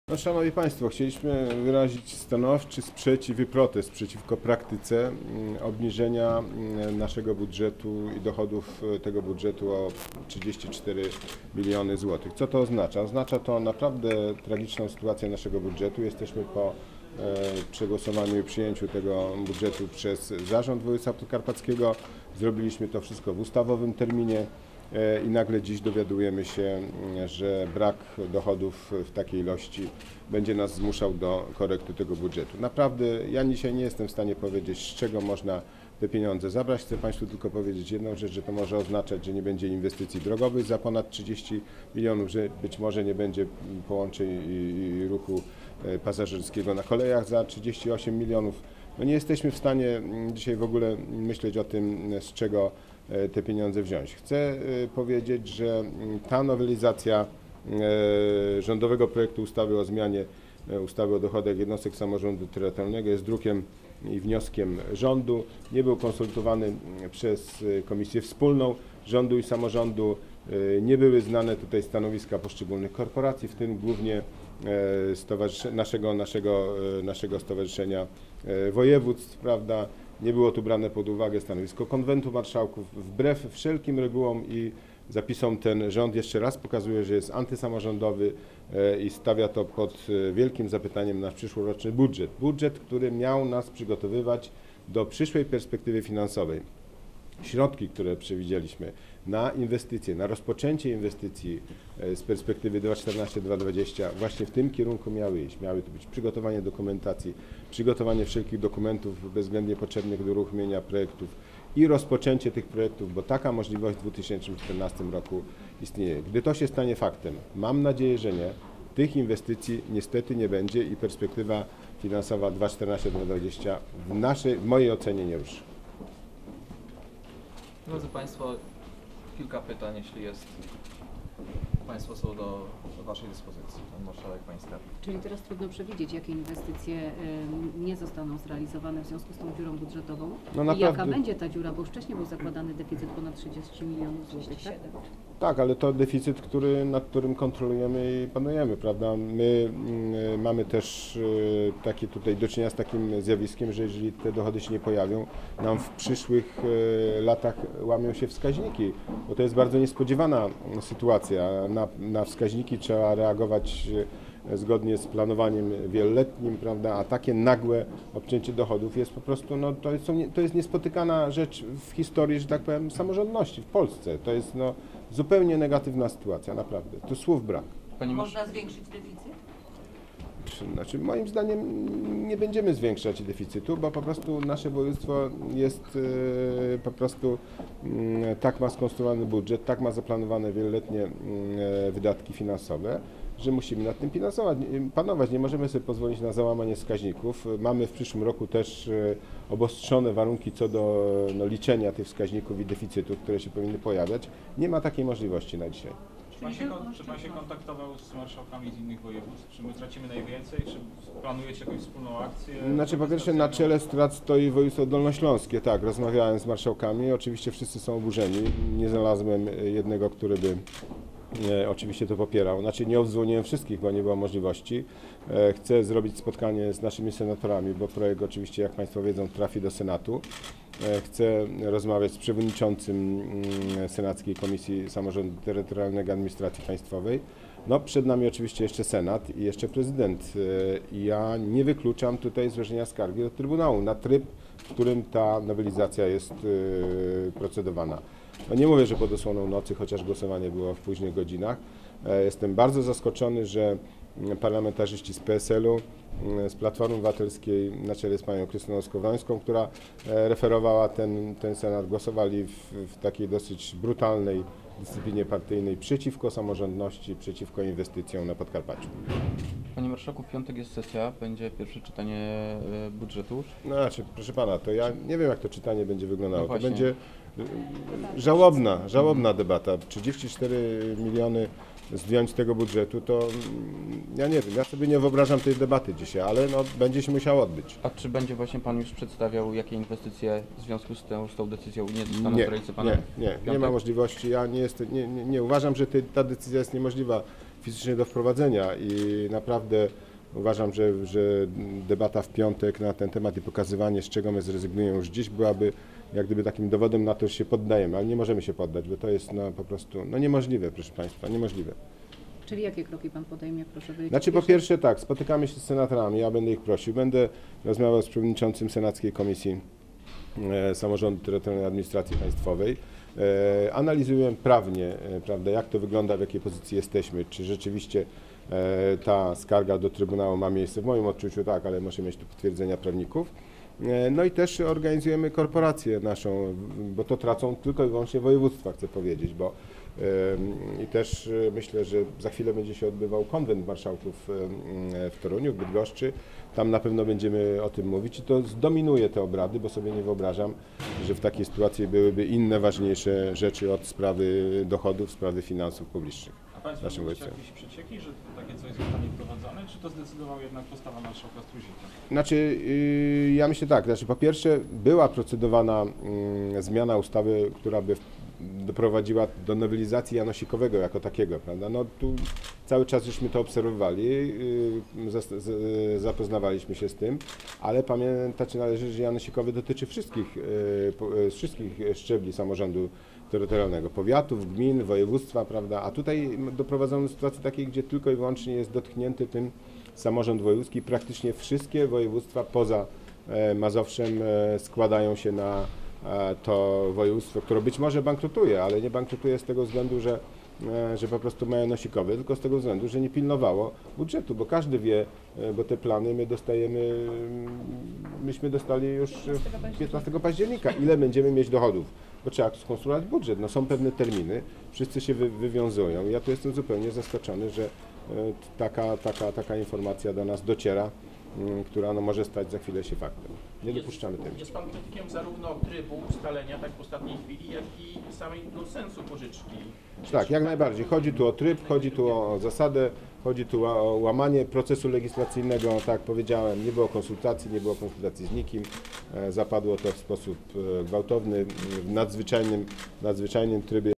Marszałek Władysław Ortyl o "janosikowym"
To tragiczna sytuacja dla budżetu województwa podkarpackiego -- tak o decyzji posłów, którzy przegłosowali zmiany w tzw. janosikowym powiedział dzisiaj marszałek Władysław Orty dodając, że ta decyzja oznacza zmniejszenie dochodów samorządu o 34 mln złotych. To tragedia dla naszego budżetu, szczególnie, że jest to niespodziewana decyzja, która nie była z nami konsultowana. Takie praktyki nigdy nie miały miejsca w historii samorządności -- powiedział marszałek dodając, że proponowane przez posłów zmiany oznaczają drastyczną korektę budżetu i rezygnację z inwestycji lub zadań realizowanych przez samorząd.